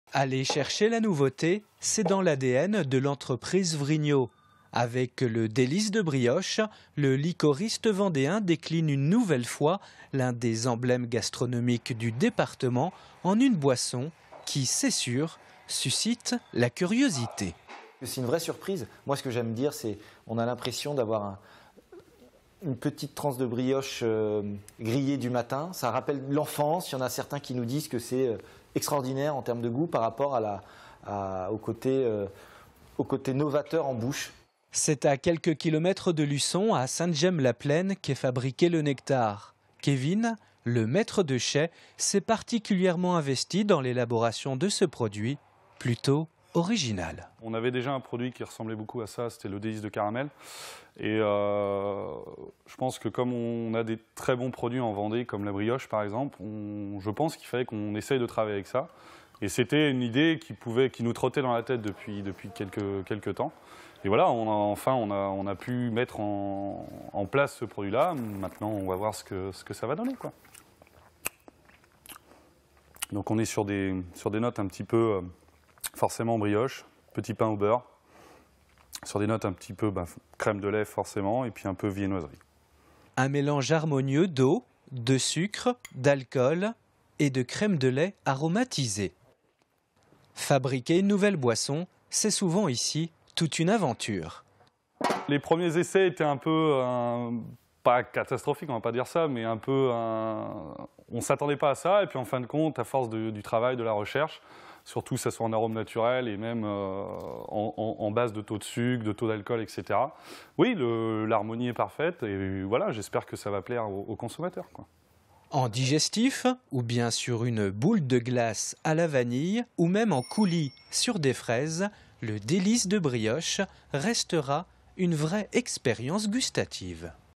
Luçon : La Maison Vrignaud lance une liqueur de brioche
Sites webs/blogs, copiez cette vidéo avec le bouton "Intégrer" !
 Direction Luçon pour découvrir la liqueur de brioche avec l'entreprise Vrignaud, qui détourne la traditionnelle brioche de Vendée en boisson.